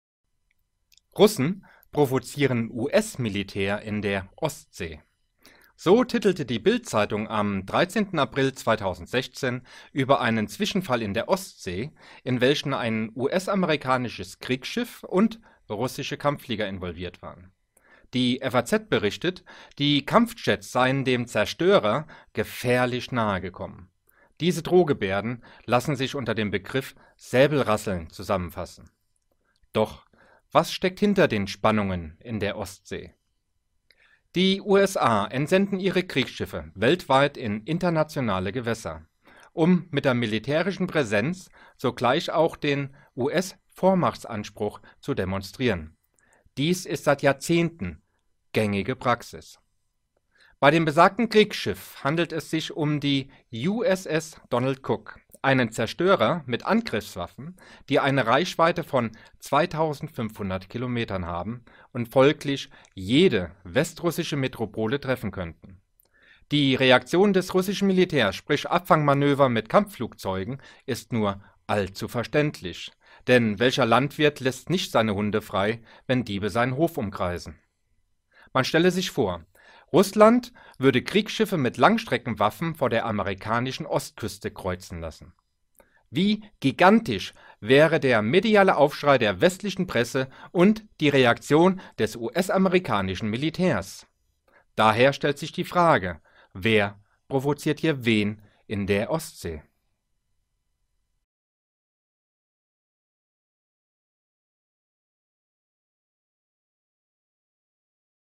Vapenskrammel i Östersjön
”Ryssarna provocerar US-militären i Östersjön” Det var rubriken som stod den 13:e april 2016 i den tyska tidningen BILD.